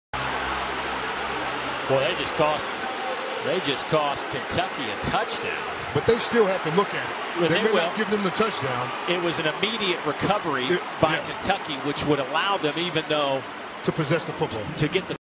Mark Stoops is LIVID 😤😤😤 After refs cost Kentucky a touchdown
Kentucky gets off to a 1-0 start with a 44-14 win over Ball State